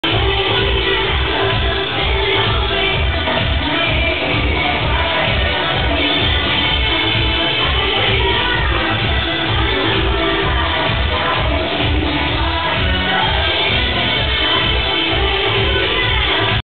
Vibeza stars
Premier stars Used at Vibes nightclub bristol before it closed in 2008